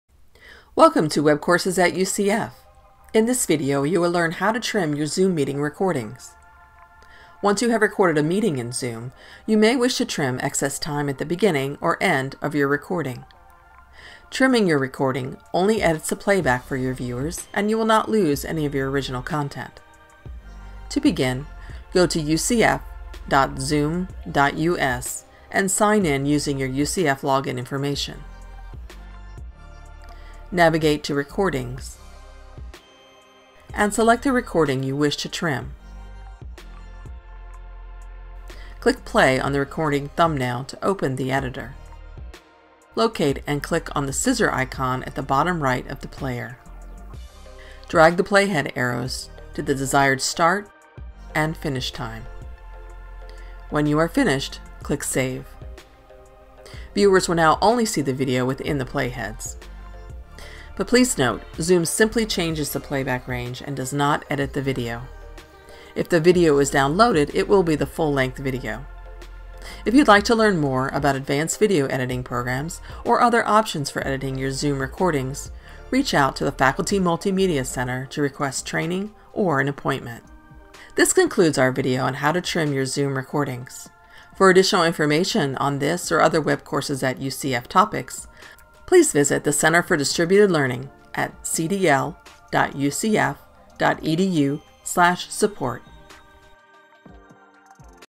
How to Trim Your Zoom Recording
Learn how to trim your Zoom Meeting Recordings.